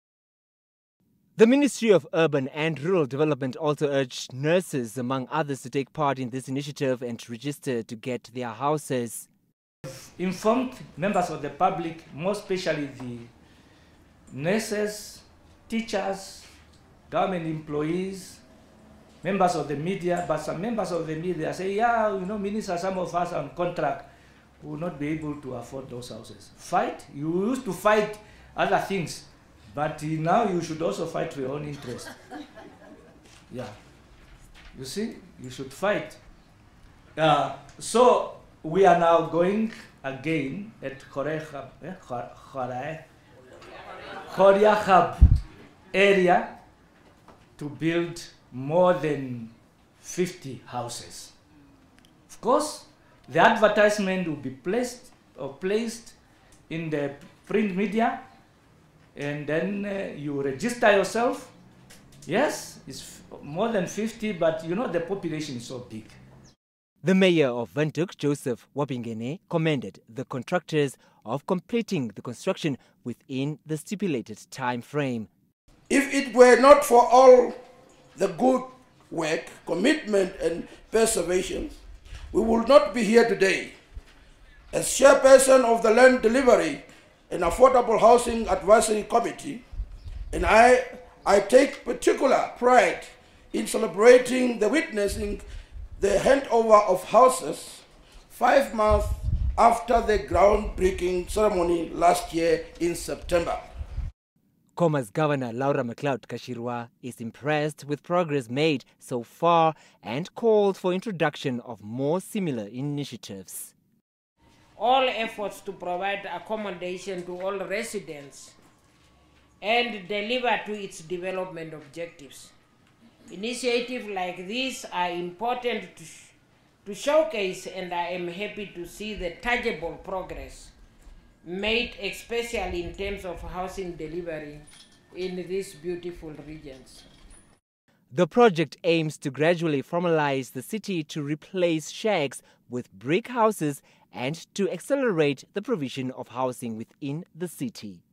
Minister Uutoni hands over eight houses under the Affordable Housing Pilot Programme - nbc
The Minister of Urban and Rural Development, Erastus Uutoni has handed over eight houses under the Affordable Housing Pilot Programme of the Windhoek Municipality.
Uutoni encouraged the councillors to work together regardless of their political differences.